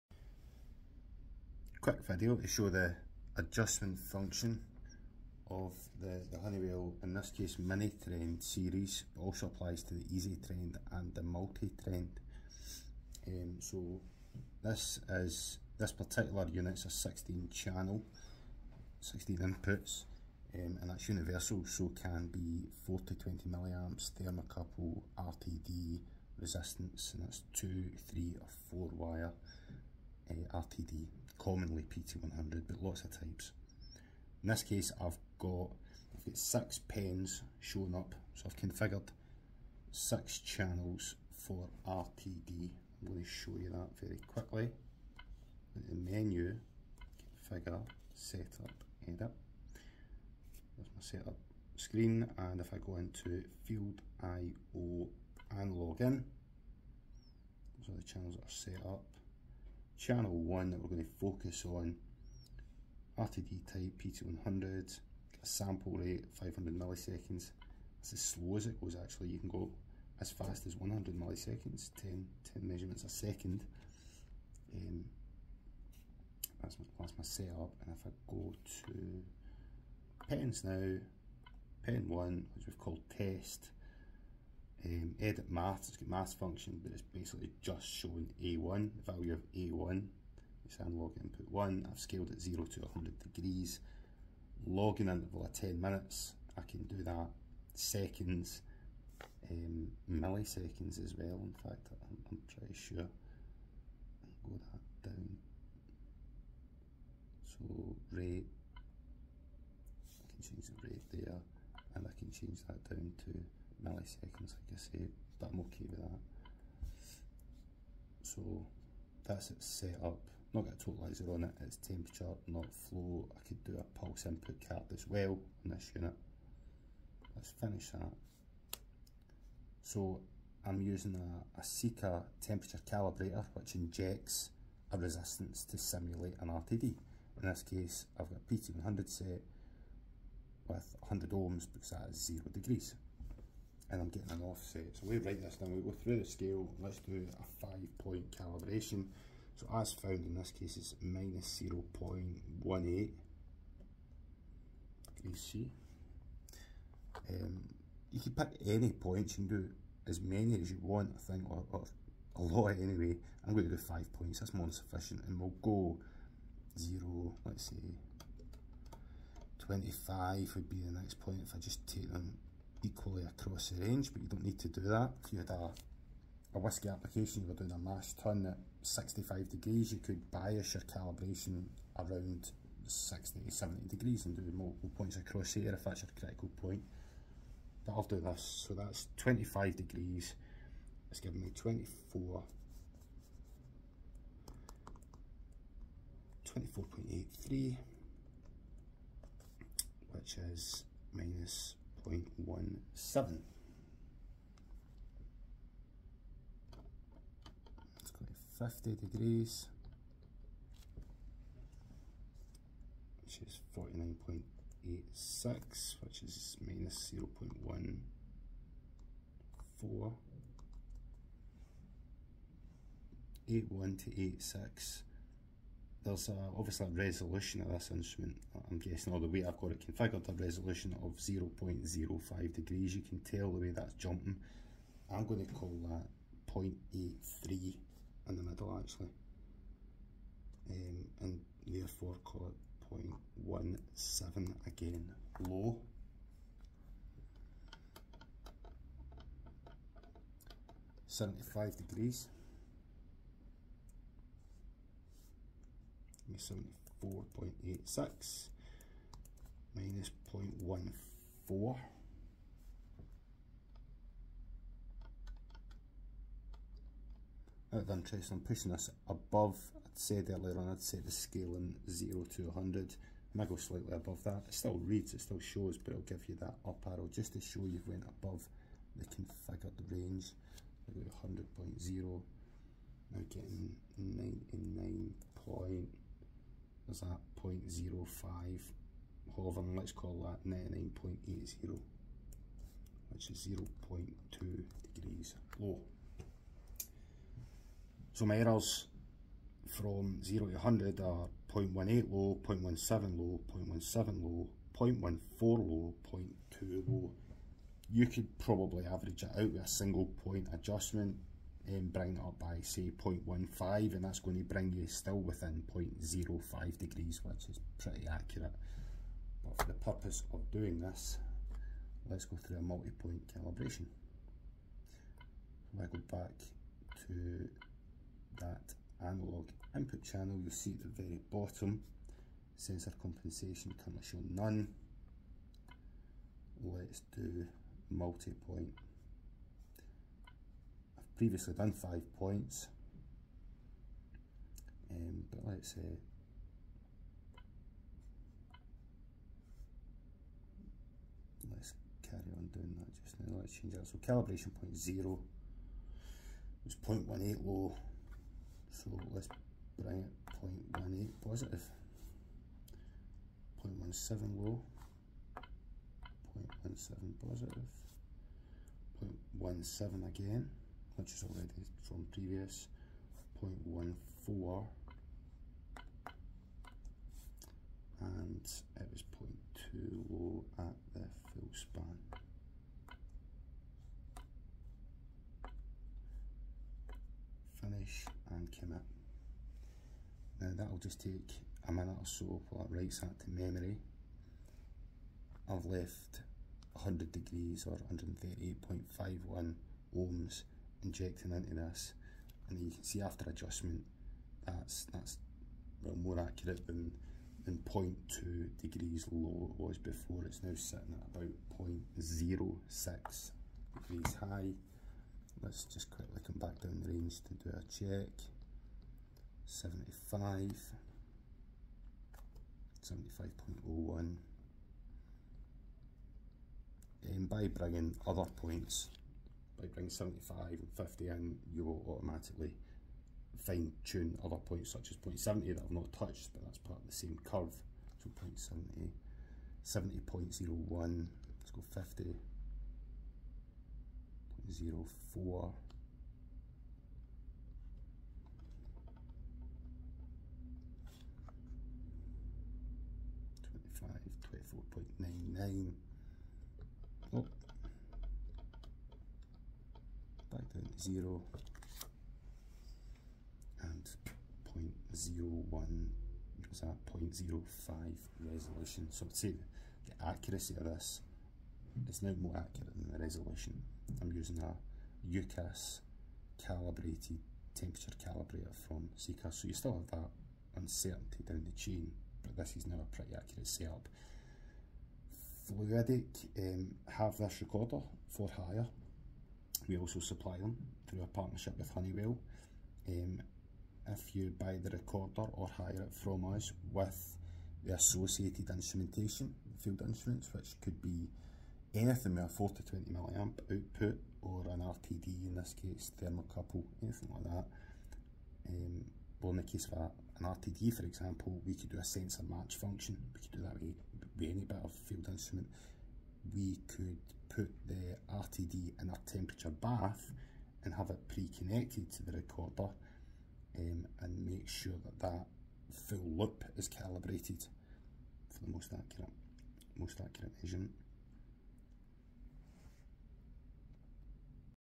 Calibrating a Honeywell Trend series paperless recorder
Configuration of a Honeywell MiniTrend paperless recorder for RTD, Pt100 input range 0-100C with 5 point calibration and adjustment for best accuracy performance.  fluidic hire and supply Honeywell recorders and can configure and calibrate for your application.